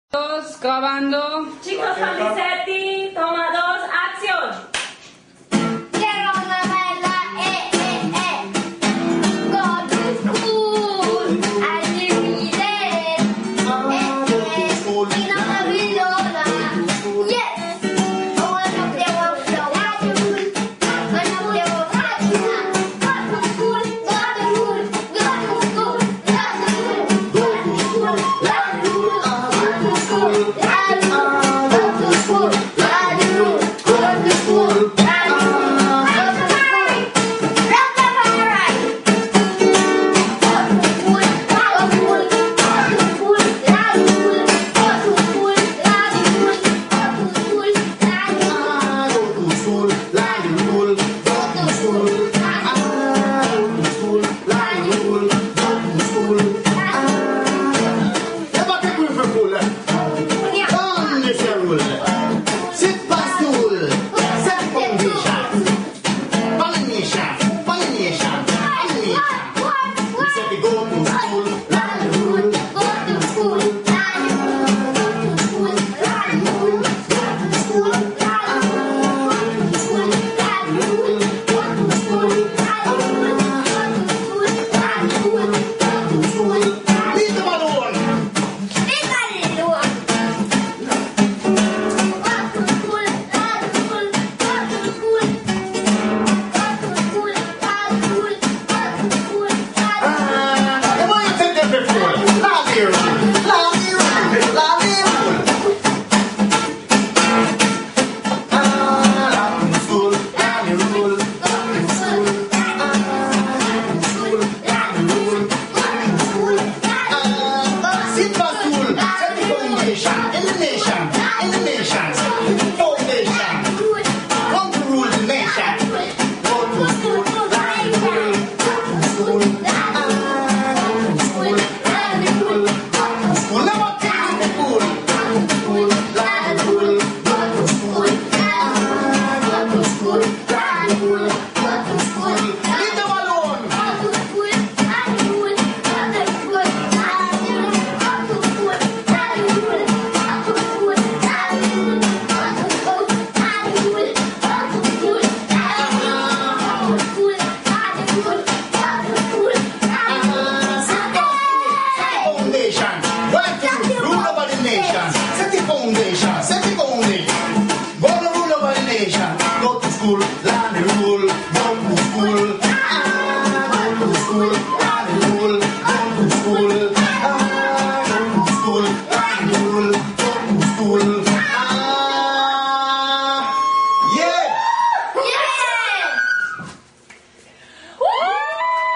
Robert Salomon & Isaac Anthony - Live in Settee 2011.
Robert Salomon and Isaac Anthony Live Show at Setee Foundation 2011, Original Texts of the song by Robert Salomon and Isaac Anthony